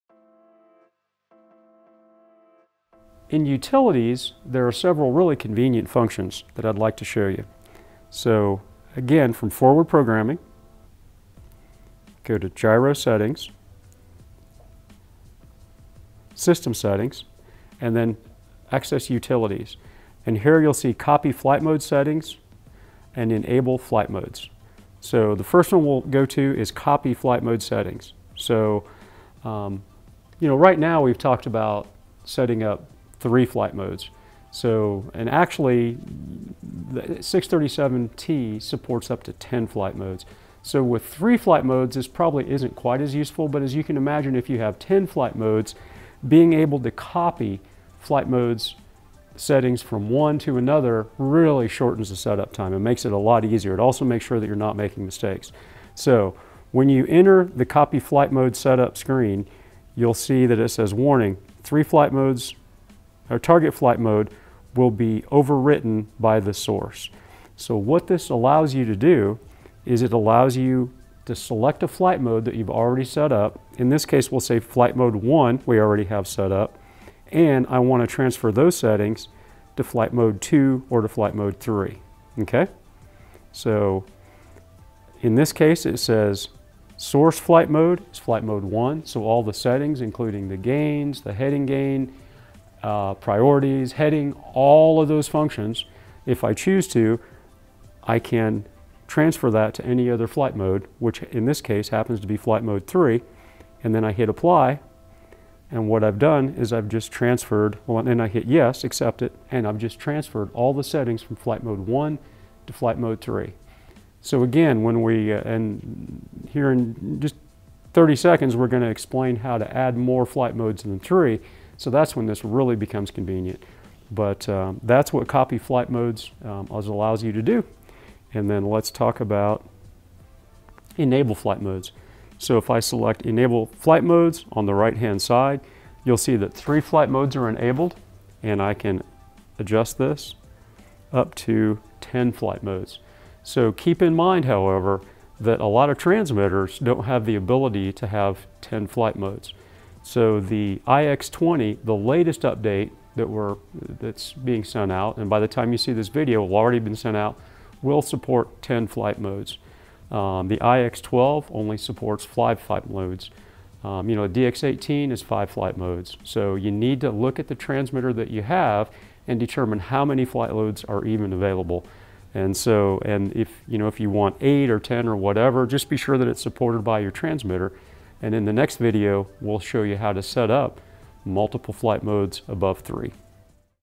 Spektrum Smart Receiver AS3X Setup Series #17 - Utilities Menu
Join fellow RC enthusiast and Spektrum Technical Director John Adams for our simple to follow setup tutorial series. It is our goal in this series to quickly and simply instruct RC pilots how to use the more advanced settings for AS3X on the AR637T.

In this video, John explains the utilities menu and its options found on the AR637T.

- Please note - These instructions apply to setting up the AR637T, AR637TA (must be unlocked) AR630, AR631, AR8360T and AR10360T AS3X Safe receiver.

-Advanced AS3X Setup-